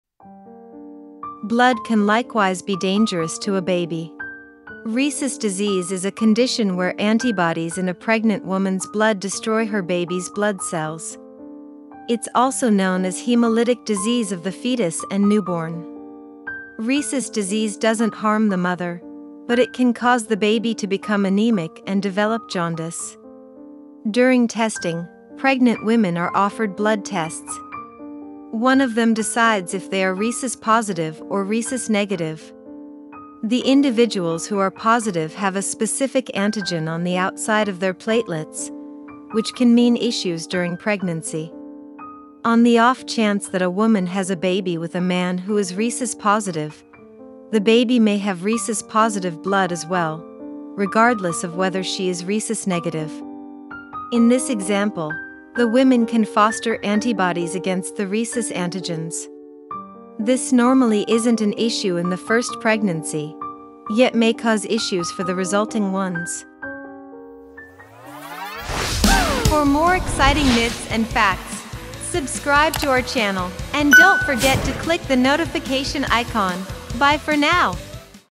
Rhesus Disease Understanding | Pregnancy Facts | mababycare
Rhesus disease is a condition where antibodies in a pregnant woman's blood destroy her baby's blood cells. It's also known as hemolytic disease of the fetus and newborn (HDFN). Rhesus disease doesn't harm the mother, but it can cause the baby to become anemic and develop jaundice.

During testing, pregnant women are offered blood tests. One of them decides if they are "rhesus positive" or "rhesus negative." The individuals who are positive have a specific antigen on the outside of their platelets, which can mean issues during pregnancy.

On the off chance that a woman has a baby with a man who is rhesus positive, the baby may have rhesus positive blood as well, regardless of whether she is rhesus negative. In this example, the women can foster antibodies against the rhesus antigens. This normally isn't an issue in the first pregnancy, yet may cause issues for the resulting ones.

Animation is created by mababycare